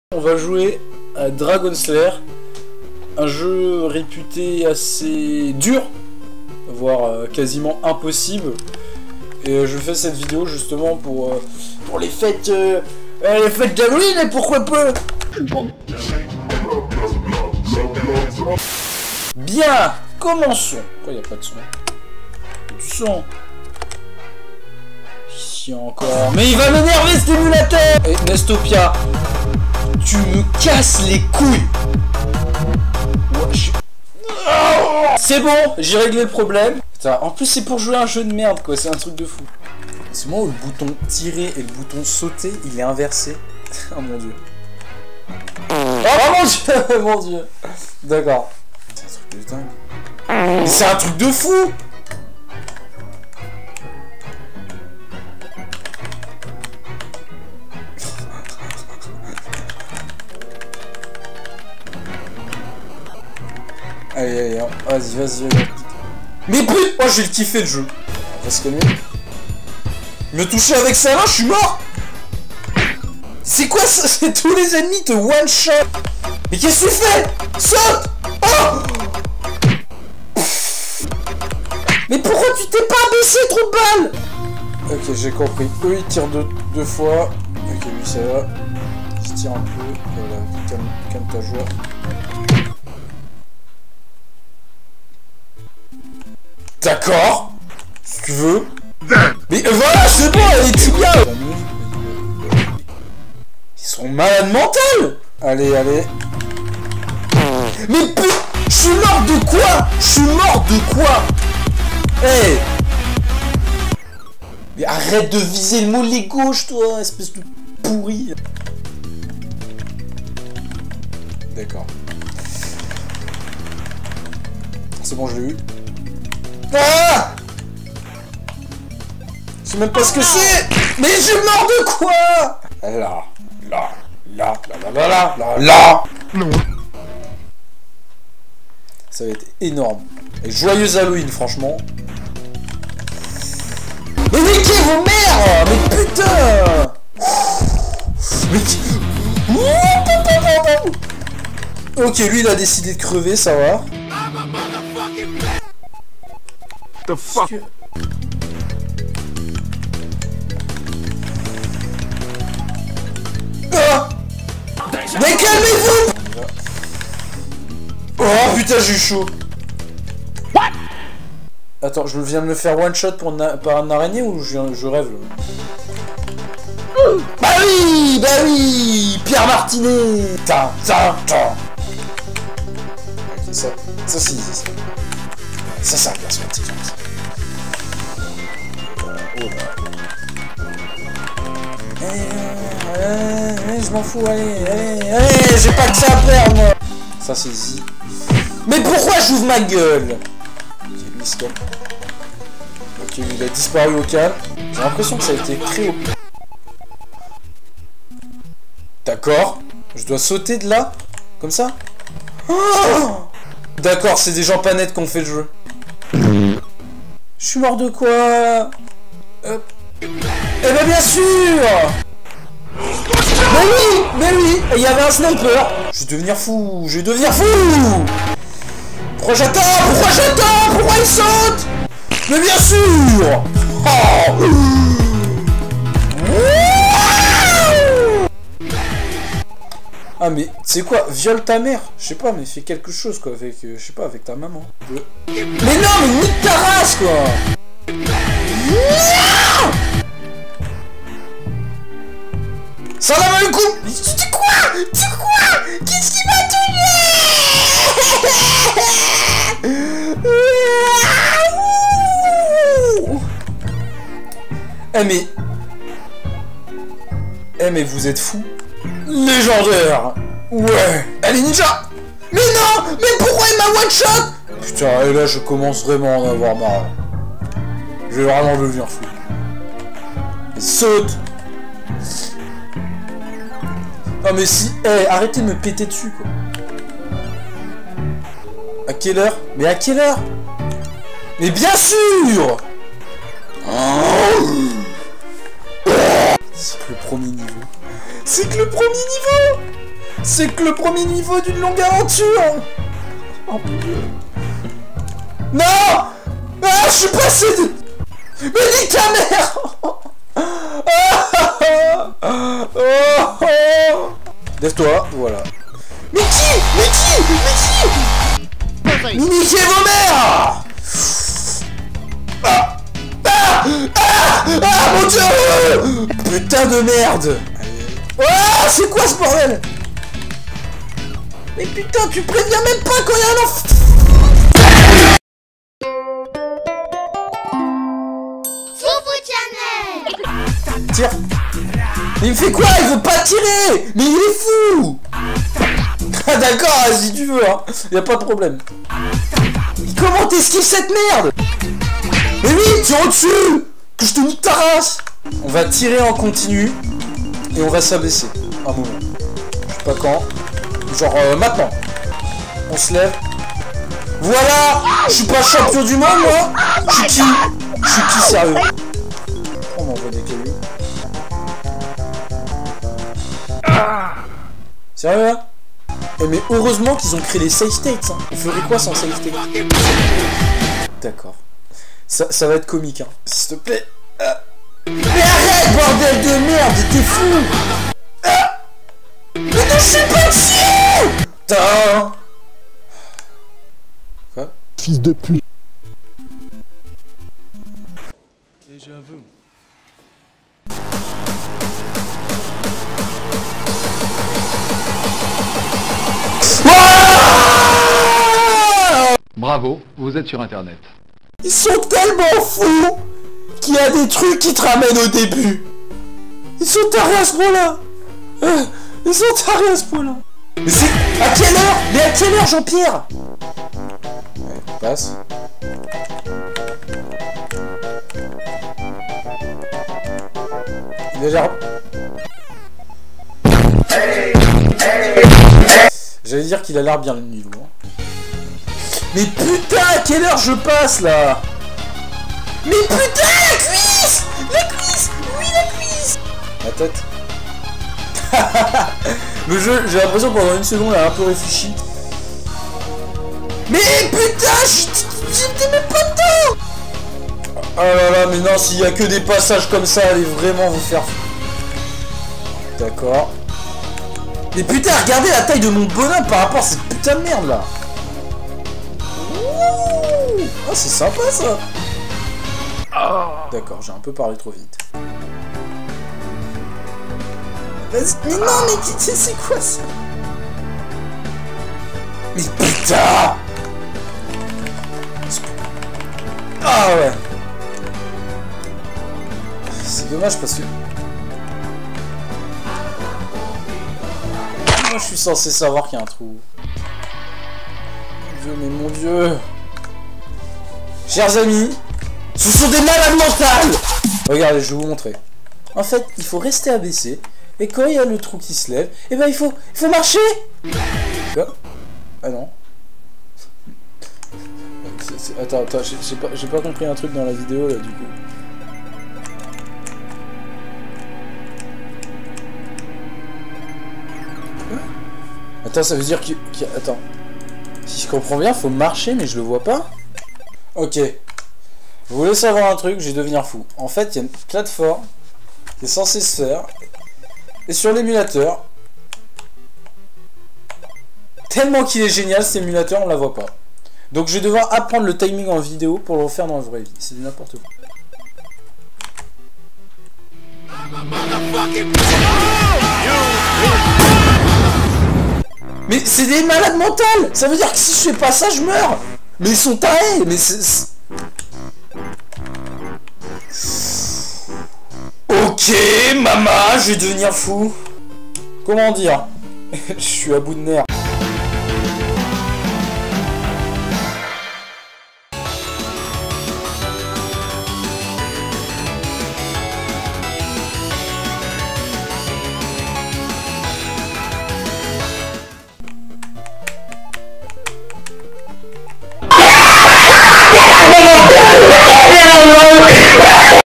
JE HURLE !
Aujourd'hui je joue à ce putain de Dragon sdfsdfsZ465JnrtzEMKgm
de merde. Je viens d'apprendre pour la "lampe". il aura peut être une partie 2, mais en tout cas c'est pas avant très longtemps, mais bon je reste motivé!